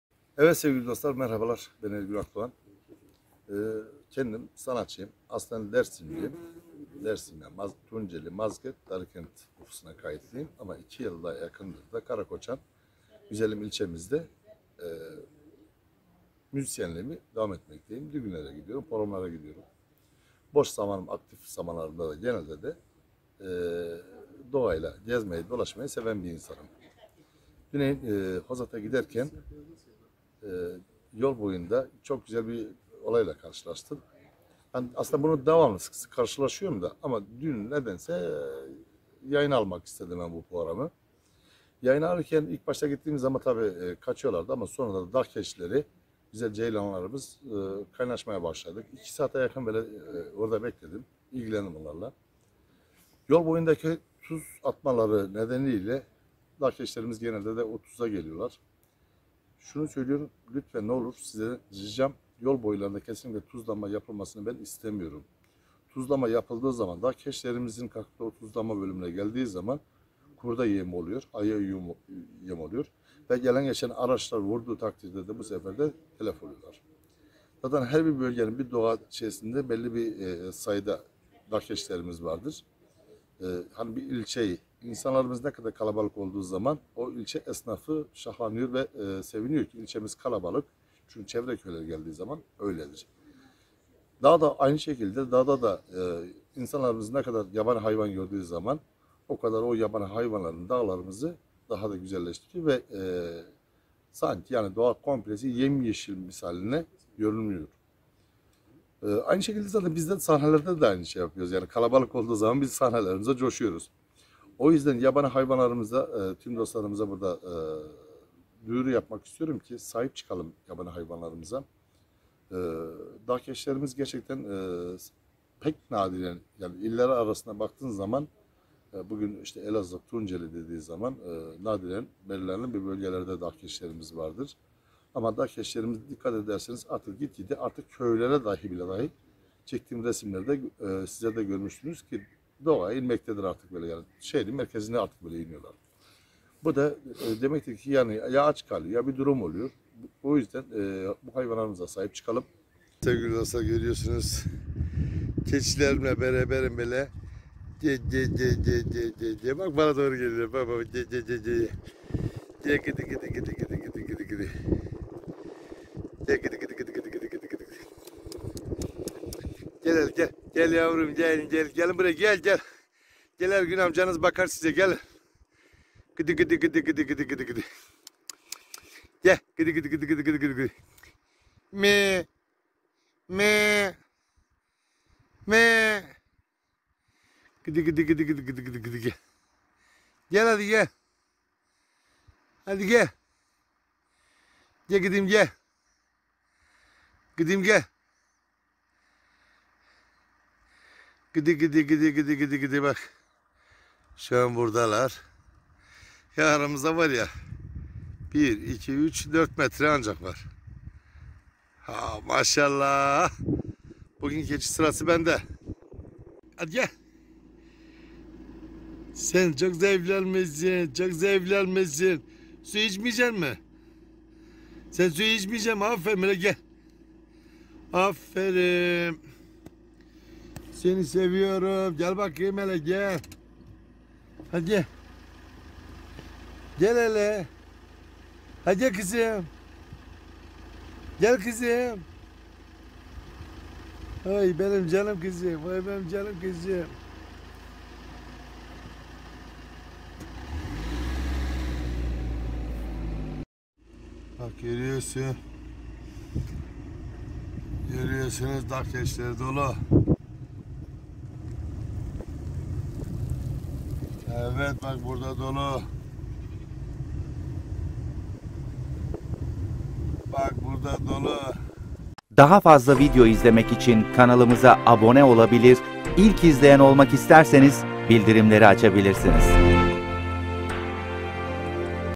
Yolda gördüğü dağ keçileri ile diyaloğu ile gündem olmuştu, o anları anlattı
Tunceli’de tuz ihtiyaçlarını gidermek için karayoluna kadar inen dağ keçileriyle yaptığı sohbetin ardından gündem olan Ergün Akdoğan, o anları anlattı.